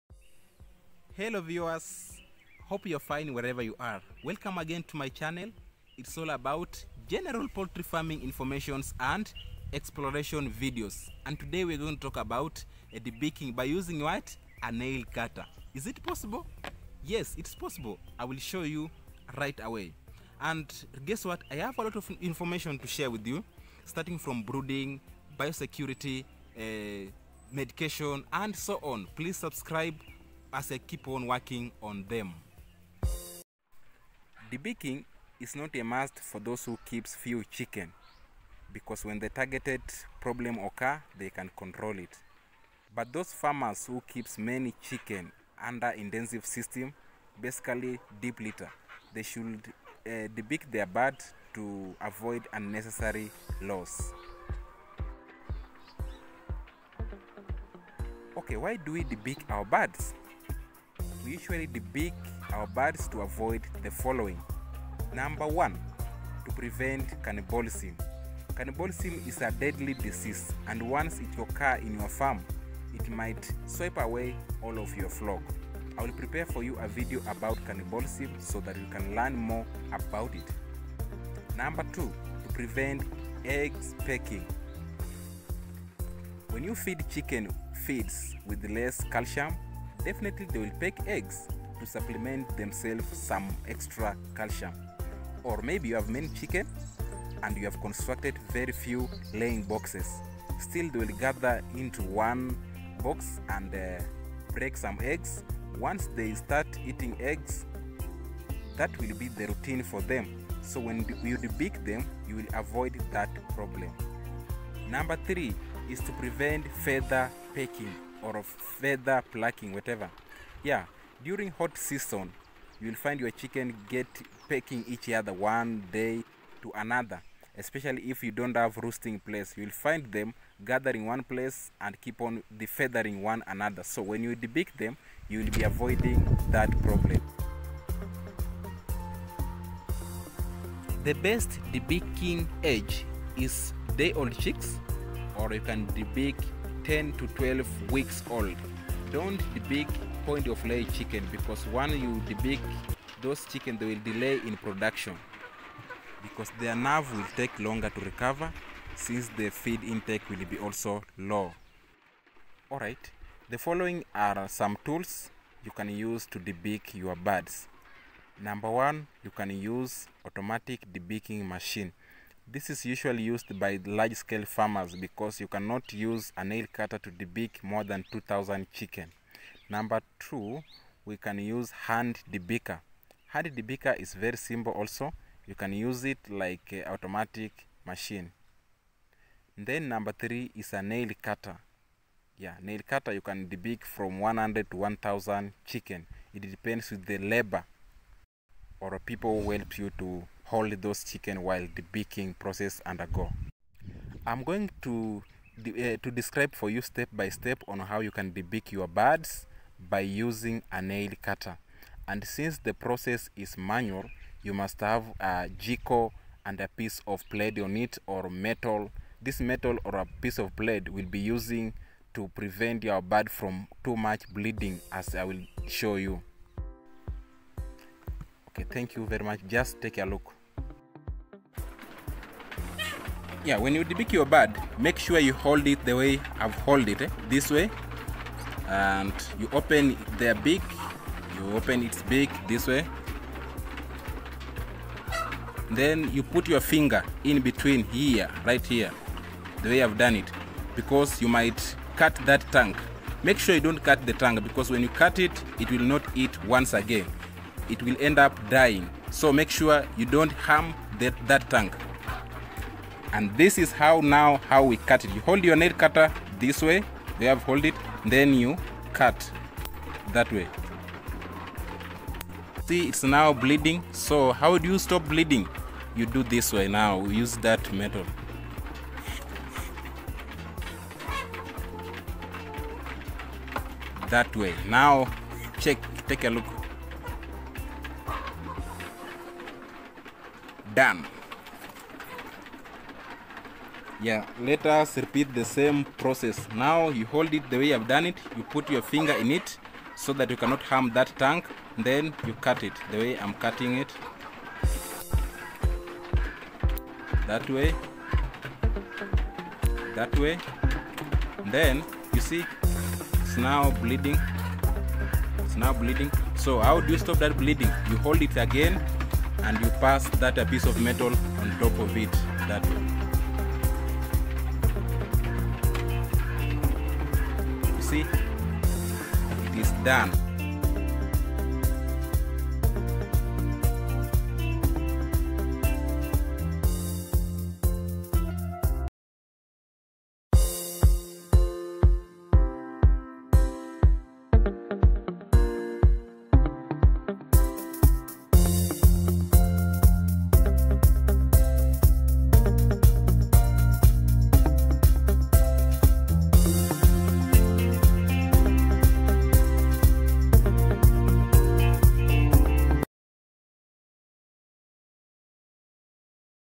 DEBEAKING BY USING NAIL CUTTER.
DEBEAKING

-We usually Debeak to:

1.prevent eggs pecking
2.To prevent  Cannibalism
3.To prevent feather pecking